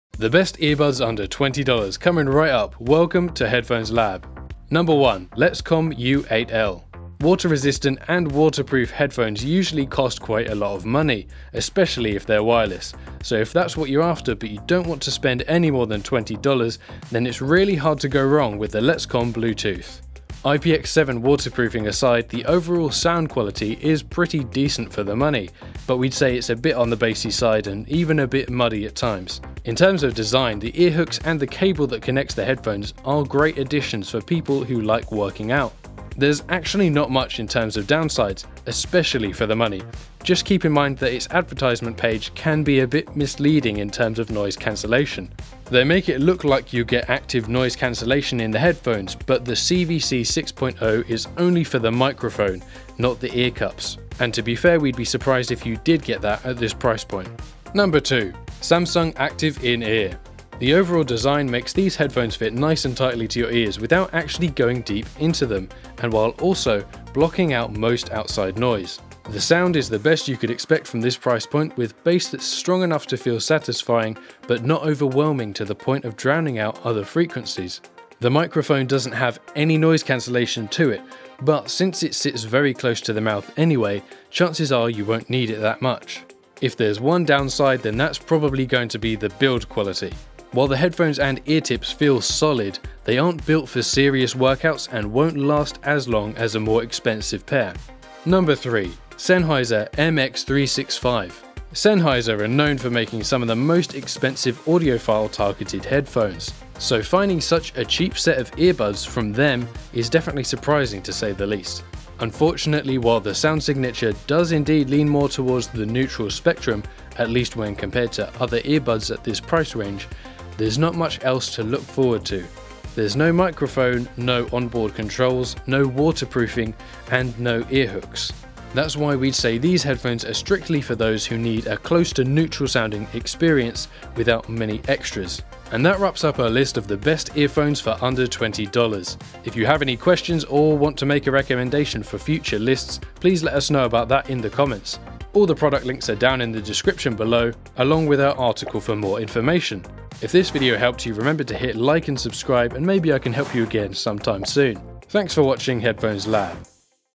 Best Earbuds under $20 of 2021!
Welcome to Headphones Lab! In this video, we will be showing you the best earbuds under $20 of 2021.

🔍 Find The Best Headphones Here:
LETSCOM U8L Bluetooth Headphones

Samsung Active In-Ear buds

Sennheiser MX 365 Earbuds

📖 Transcript:

The best earbuds under $20, coming right up - Welcome to Headphones Lab!

LETSCOM U8L

Water-resistant and waterproof headphones usually cost quite a lot of money – especially if they are wireless. So, if that’s what you’re after but you don’t want to spend any more than $20, then it’s really hard to go wrong with the LETSCOM Bluetooth!

IPX7 Waterproofing aside, the overall sound quality is pretty decent for the money. We’d say it’s a bit on the bassy side and even a bit muddy at times.

In terms of design, the ear-hooks and the cable that connects the headphones are great additions for people who like working out. After all, you won’t have to worry about losing the earbuds while jogging.

There’s certainly not much in terms of downsides. Especially for the money. Just keep in mind that its advertisement page can be a bit misleading in terms of noise cancelation – They make it look like you’re getting actual noise cancelation, but, CVC 6.0 is only for the microphone, not the ear cups, and we’d be surprised if you did get that at that price point.

Samsung Active In-Ear

The overall design makes these headphones fit nice and tightly to your ears without actually going deep into them while also blocking most of the outside noises.

The sound is the best you can expect from this price point, with bass that’s strong enough to feel satisfying but not overwhelming to the point of drowning out the other frequencies.

The microphone doesn’t have any noise cancelation to it. But, since it sits very close to the mouth anyway, chances are you won’t need it that much.

If there’s one downside, then that’s probably going to be the build quality. While the headphones and the ear tips feel solid, they aren’t built for serious workouts and won’t last as long as a more expensive pair.

Sennheiser MX 365

known for making some of the most expensive, audiophile-targeted headphones. So, finding such a cheap set of earbuds for them is definitely surprising, to say the least.

Unfortunately, while the sound signature does indeed lean more towards the neutral spectrum (At least when compared to other earbuds at this price range), there’s not much else to look forward to. There is no microphone, no onboard controls, no waterproofing, and no ear hooks.

That’s why we’d say these headphones are strictly for those who need a close to neutral-sounding experience without much else.